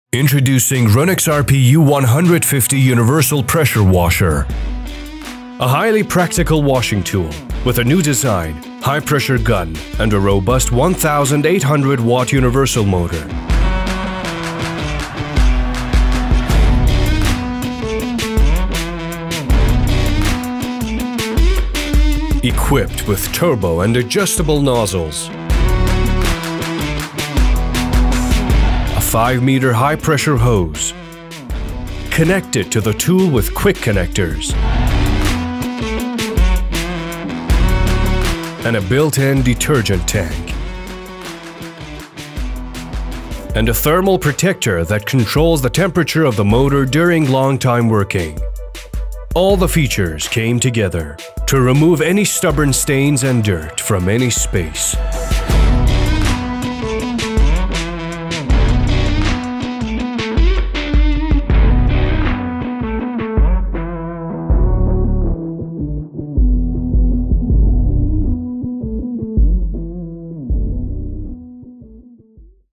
🚀 Ronix RP U150 The Secret Weapon for Ultimate Cleanliness 🛡️
Get ready to be blown away by the cleaning power of the Ronix RP-U150 Universal High-Pressure Washer! 🌪️

Join us in this video as we dive into a world of sparkling clean surfaces, from dirty driveways to grime-covered vehicles. We'll showcase the incredible features of the RP-U150 and offer an authentic review that will make you want to grab one for yourself. 🚿

With its 1800 w universal motor, say goodbye to the mess and hello to the magic of high-pressure cleaning. 🚀 Watch now to witness the transformation and discover why the RP-U150 is the beast you need to conquer dirt and grime. 🌟